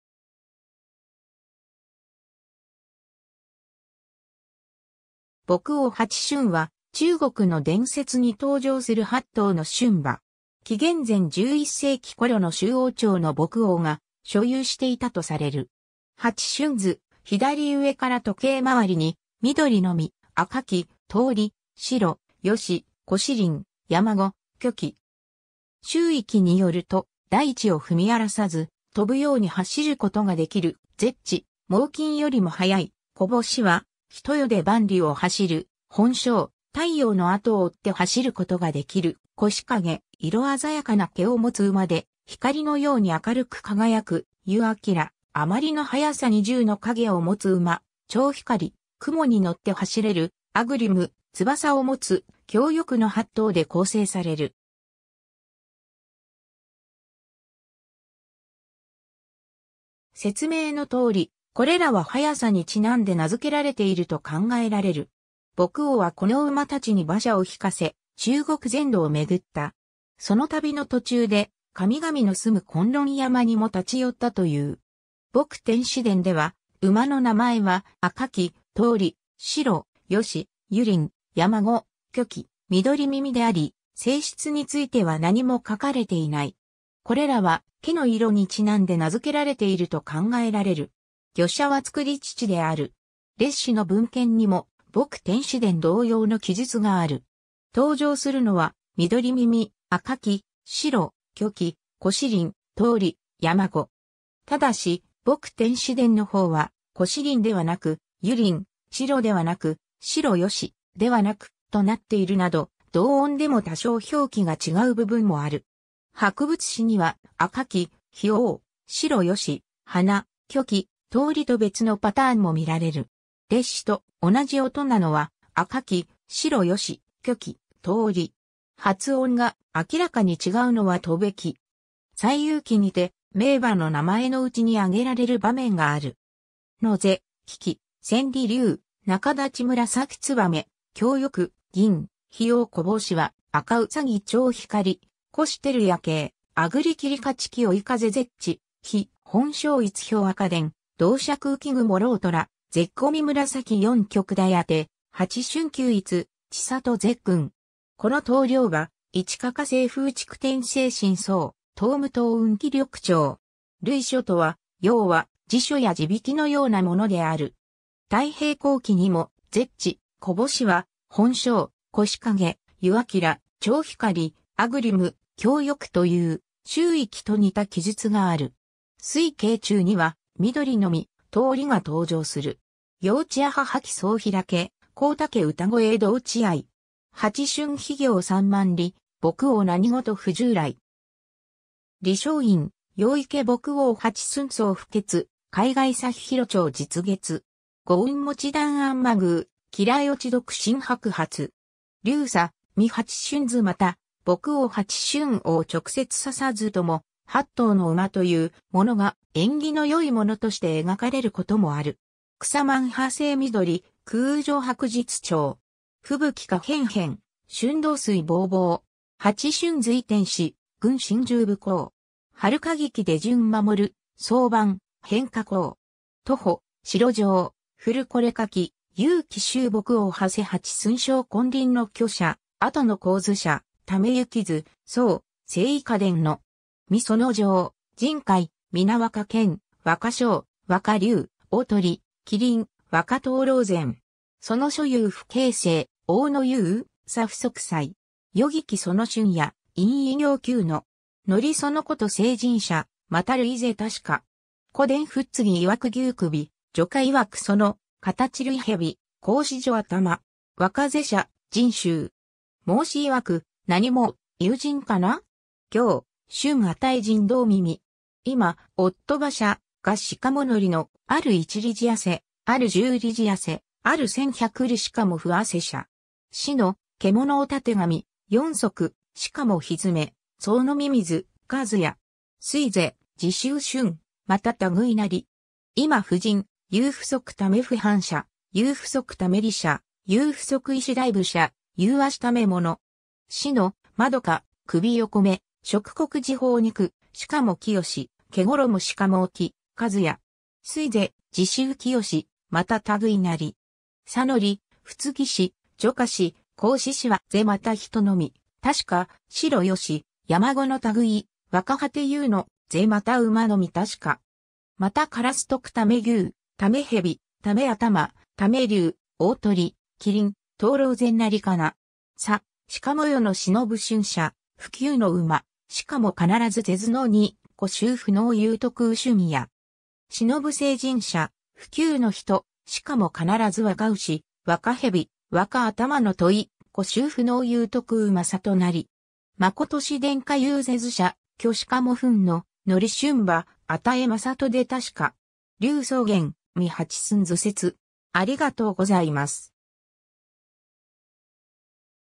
穆王八駿
神話・伝説の馬
穆王八駿（ぼくおうはっしゅん）は、中国の伝説に登場する8頭の駿馬。
 紀元前11世紀頃の周王朝の穆王が所有していたとされる。
 八駿図：左上から時計回りに驊騮、緑耳、赤驥、盗驪、白義、踰輪、山子、渠黄。
(漢字儘) 『拾遺記』によると、大地を踏み荒らさず、飛ぶように走ることができる「絶地（ぜっち）」、猛禽（鳥）よりも速い「翻羽（ほんう）」、一夜で万里(5000km?)を走る「奔霄（ほんしょう）」、太陽の後を追って走ることができる「越影（えつえい）」、色鮮やかな毛を持つ馬で、光のように明るく輝く「逾輝（ゆき）」、余りの速さに10の影を持つ馬「超光（ちょうこう）」、雲に乗って走れる「騰霧（とうむ）」、翼を持つ「挟翼（きょうよく）」の8頭で構成される。
説明の通り、これらは速さにちなんで名づけられていると考えられる。
 穆王はこの馬達に馬車を引かせ、中国全土を巡った。
その旅の途中で、神々の住む崑崙山にも立ち寄ったという。
 『穆天子伝』では馬の名前は「赤驥（せきき）」、「盗驪（とうれい）」、「白義」、「逾輪（ゆりん）」、「山子」、「渠黄（きょこう）」、「驊騮（かりゅう）」、「緑耳」であり、性質については何も書かれていない。
これらは、毛の色にちなんで名づけられていると考えられる。
御者は造父である。
 『列子』の文献にも『穆天子伝』同様の記述がある。
登場するのは「𦽊騮」「緑耳」「赤驥」「白𣚘（はくぎ）」「渠黄」「踰輪」「盗驪」「山子」。
ただし、『穆天子伝』の方は、「踰輪」ではなく「逾輪」(yú lún)、「白𣚘」ではなく「白義」(bái yì)、「𦽊騮」ではなく「驊騮」(huá liú)となっているなど、同音でも多少表記が違う部分もある。
 『博物志』には「赤驥」「飛黄」「白義」「華騮」「騄駬」「騧騟」「渠黄」「盗驪」と別のパターンも見られる。
『列子』と同じ音なのは赤驥、白義、驊騮、騄駬、渠黄、盗驪。
発音が明らかに違うのは飛黄、騧騟。
 西遊記にて、名馬の名前の内に挙げられる場面がある。
 乃是：驊騮騏驥，騄駬繊離；龍媒紫燕，挟翼驌驦；駃騠銀騔，騕褭飛黄；騊駼翻羽，赤兔超光；踰輝弥景，騰霧勝黄；追風絶地，飛翮奔霄；逸飄赤電，銅爵浮雲；驄瓏虎𩤲，絶塵紫驎；四極大宛，八駿九逸，千里絶群。
此等良馬，一个个嘶風逐電精神壮，踏霧登雲気力長。
 類書とは、要は辞書や字引のようなものである。
 『太平広記』にも「絶地」「翻羽」「奔霄」「越影」「逾輝」「超光」「騰霧」「挟翼」という『拾遺記』と似た記述がある。
 『水経注』には驊騮、緑耳、盗驪が登場する。
 瑶池阿母綺窗開，黄竹歌声動地哀。
八駿日行三万里，穆王何事不重来。
 — 李商隠、瑶池 穆王八駿走不歇，海外去尋長日月。
五雲望断阿母宮，帰来落得新白髪。
 — 劉叉、観八駿図 また、穆王八駿を直接指さずとも、8頭の馬というものが縁起の良いものとして描かれることもある。
 草満巴西緑，空城白日長。
風吹花片片，春動水茫茫。
 八駿随天子，群臣従武皇。
遥聞出巡守，早晩遍遐荒。
 — 杜甫、城上 古之書有記周穆王馳八駿升昆侖之墟者，後之好事者為之図，宋、斉以下伝之。
観其状，甚怪，咸若騫，若翔，若龍、鳳、麒麟，若螳螂然。
其書尤不経世多有，然不足采。
世聞其駿也，因以異形求之。
則其言聖人者，亦類是矣。
故伝伏羲曰牛首，女媧曰其形類蛇，孔子如倛頭。
若是者甚衆。
 孟子曰：“何以異于人哉?堯、舜与人同耳!”今夫馬者，駕而乗之，或一里而汗，或十里而汗，或千百里而不汗者。
視之，毛物尾鬣，四足而蹄，齕草飲水，一也。
推是而至于駿，亦類也。
今夫人，有不足為負販者，有不足為吏者，有不足為士大夫者，有足為者。
視之，圓首横目，食穀而飽肉，絺而清，裘而燠，一也。
推是而至于聖，亦類也。
然則伏羲氏、女媧氏、孔子氏，是亦人而已矣;驊騮、白義、山子之類，若果有之，是亦馬而已矣。
又烏得為牛、為蛇、為倛頭，為龍、鳳、麒麟、螳螂然也哉! 然而世之慕駿者，不求之馬，而必是図之似，故終不能有得于駿也。
慕聖人者，不求之人，而必若牛、若蛇、若倛頭之問，故終不能有得于聖人也。
誠使天下有是図者，挙而焚之，則駿馬与聖人出矣! — 柳宗元、観八駿図説